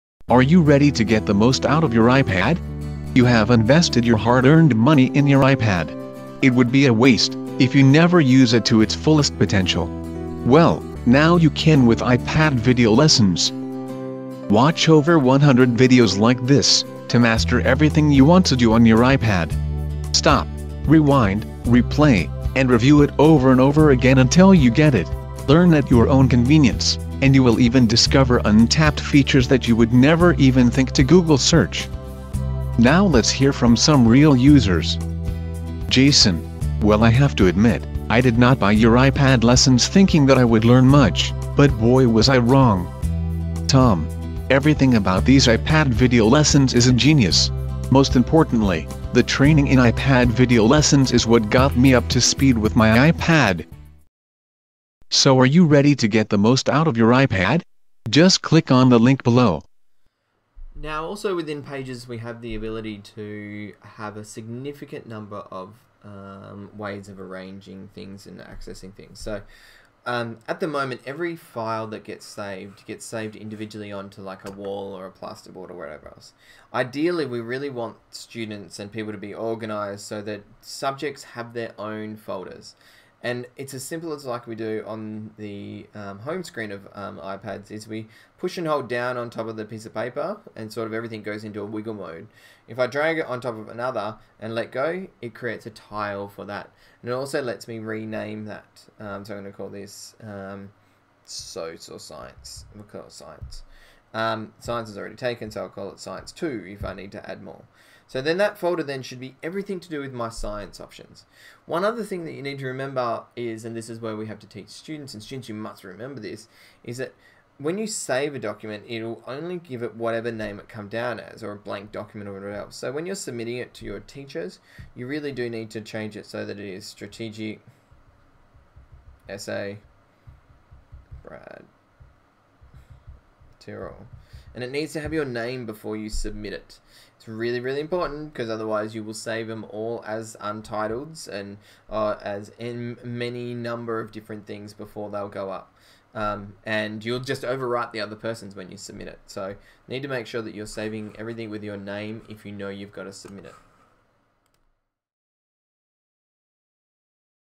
iPad 2 Tutorial - Apple iPad 2 Tutorial Getting Started
The Apple iPad is an amazing tablet, and to help you get the most out of it, we've put together a comprehensive list of every tip, trick, and tutorial.